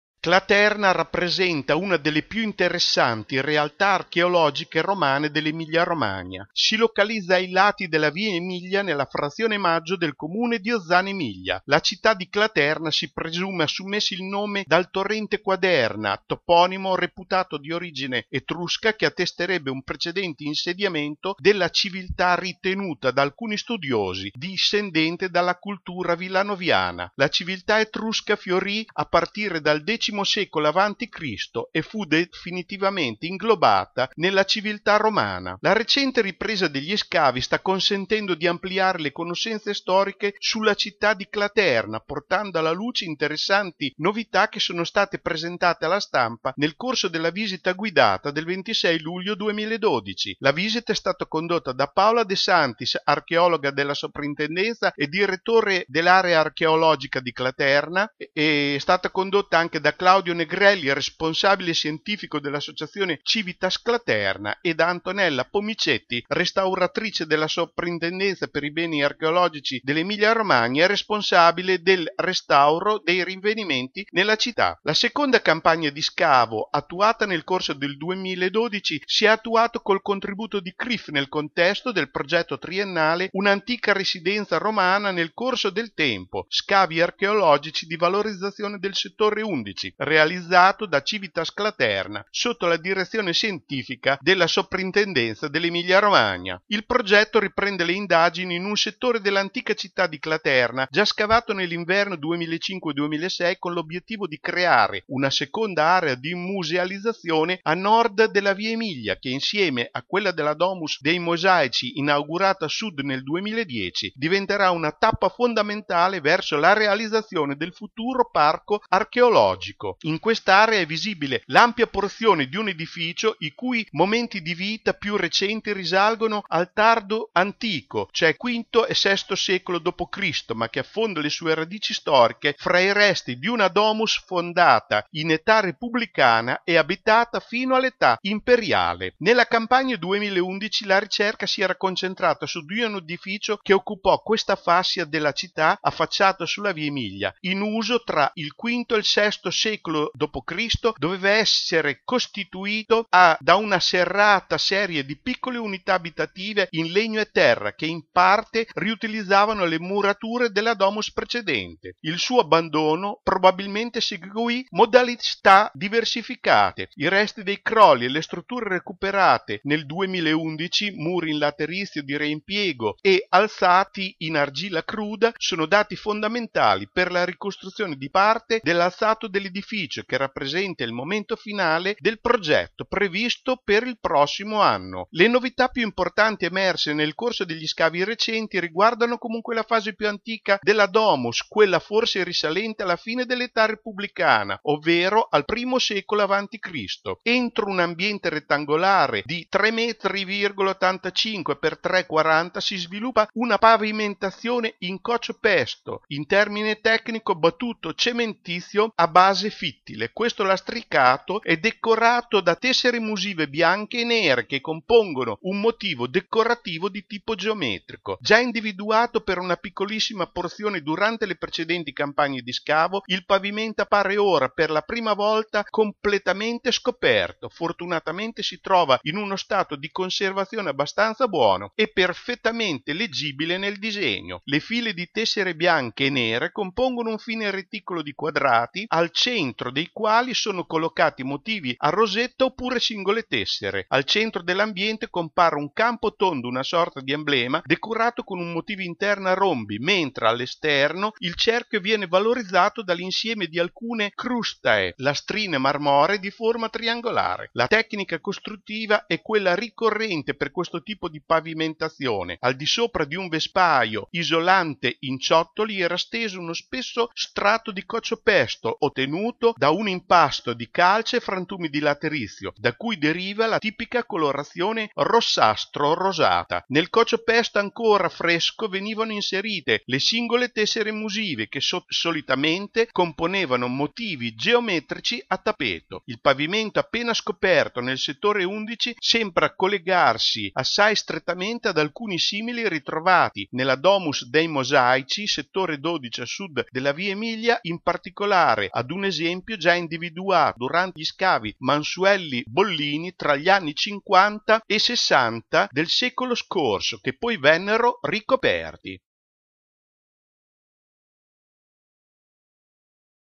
Claterna: la recente ripresa degli scavi
Claterna rappresenta una delle più interessanti realtà archeologiche romane dell'Emilia-Romagna; si localizza ai lati della via Emilia nella frazione Maggio del Comune di Ozzano Emilia. La città di Claterna si presume assumesse il nome dal torrente Quaderna, toponimo reputato di origine etrusca che attesterebbe un precedente insediamento della civiltà ritenuta da alcuni studiosi discendente dalla cultura villanoviana; la civiltà etrusca fiorì a partire dal X secolo a.C. e fu definitivamente inglobata nell'civiltà romana.
La recente ripresa degli scavi sta consentendo di ampliare le conoscenze storiche sulla città romana di Claterna, portando alla luce interessanti novità che sono state presentate alla stampa nel corso della visita guidata del 26 luglio 2012. La visita è stata condotta da Paola Desantis, Archeologa della Soprintendenza e Direttore dell'area archeologica di Claterna, Claudio Negrelli, Responsabile Scientifico dell'Associazione "Civitas Claterna", e Antonella Pomicetti, Restauratrice della Soprintendenza per i Beni Archeologici dell'Emilia-Romagna e Responsabile del restauro dei rinvenimenti nella città.
La seconda campagna di scavo attuata nel corso del 2012 si attua col contributo di CRIF nel contesto del progetto triennale "Un'antica residenza romana nel corso del tempo: scavi archeologici di valorizzazione del settore 11", realizzato dall'Associazione "Civitas Claterna" sotto la direzione scientifica della Soprintendenza per i Beni Archeologici dell'Emilia-Romagna. Il progetto riprende le indagini in un settore dell'antica città di Claterna già scavato nell'inverno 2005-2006, con l'obiettivo di creare una seconda area di musealizzazione a nord della via Emilia che, insieme a quella della "domus dei mosaici" inaugurata a sud nel 2010, diventerà una tappa fondamentale verso la realizzazione del futuro Parco Archeologico.